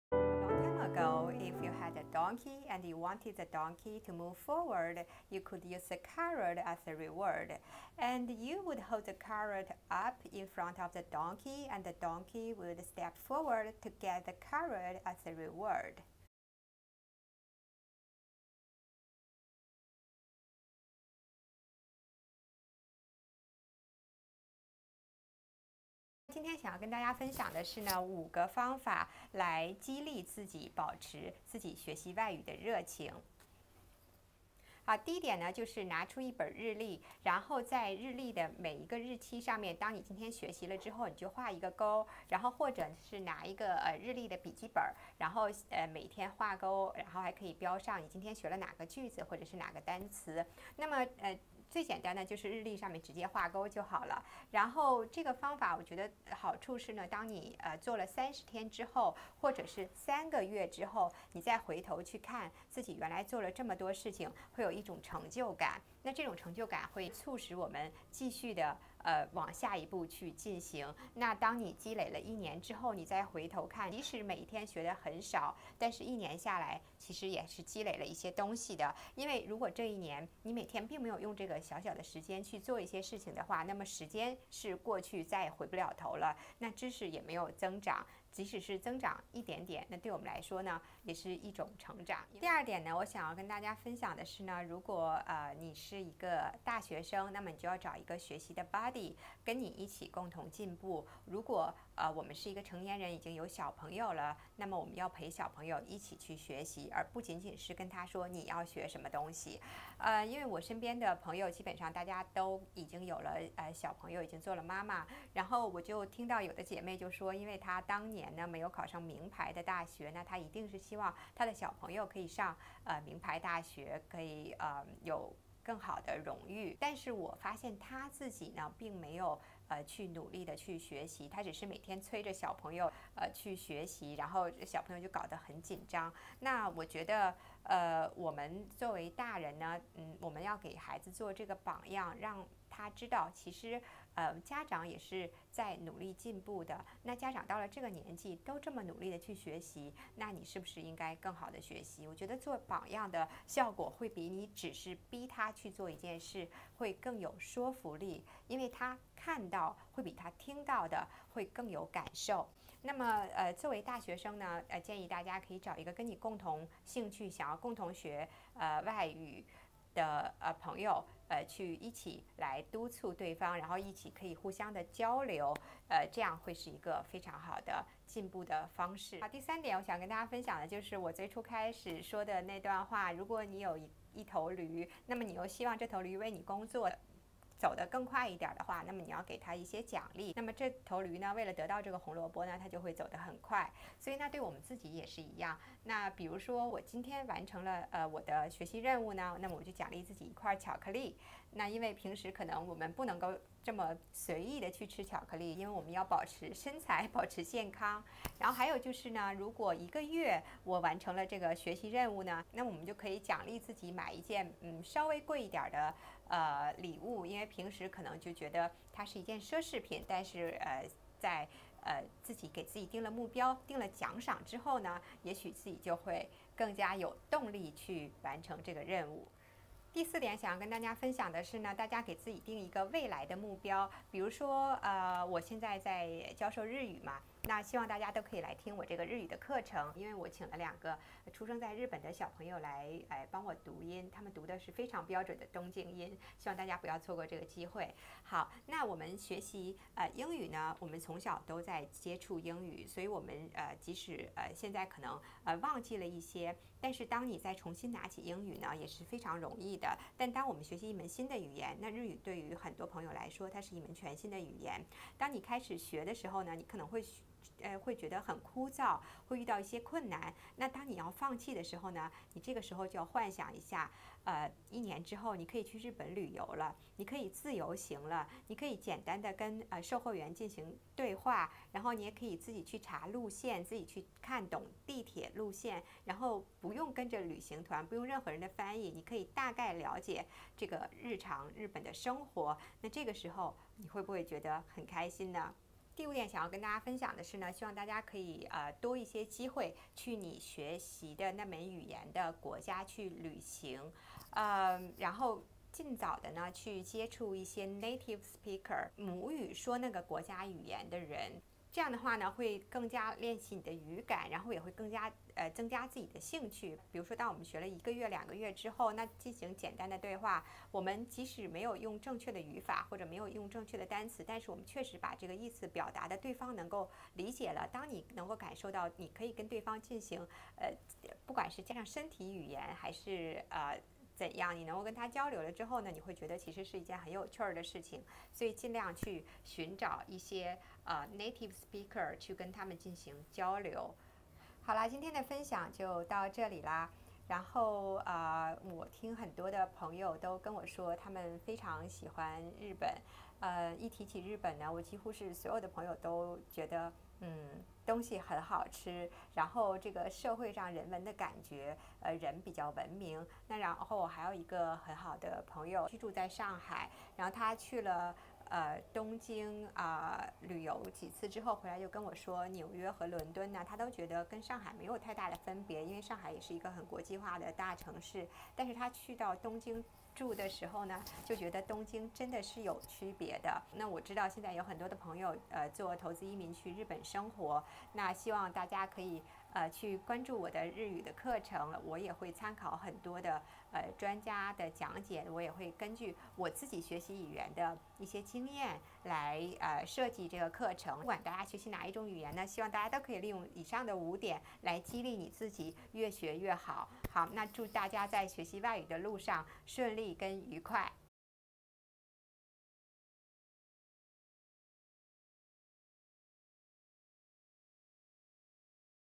如何激励自己持续学习外语
最近打算教儿子学日语，请了日本朋友在东京出生的两个小孩来带领儿子读音，发现这两个小朋友日语发音太标准了，于是决定录下来分享给大家。请大家跟着序号学习，所有课程都是跟我自己学习外语的经验设计。近几年，在校学生中，行为问题和精神问题有上升的趋势。其中熟练掌握两种语言（双语能力）的学生这些问题比例比较少。科学研究表明，这主要是由于大脑中两个语言中心平行工作并不断切换使用使得熟练掌握两种语言的学生在专注力和执行力方面较单一语言的学生更有优势。也有研究指出掌握双语或者多国语言的人在中风中恢复比较快，也可以预防老年痴呆。利用小朋友学语言黄金期，为他们积累多一个技能。大家也可以在公众号上跟着推送学习。yybhm701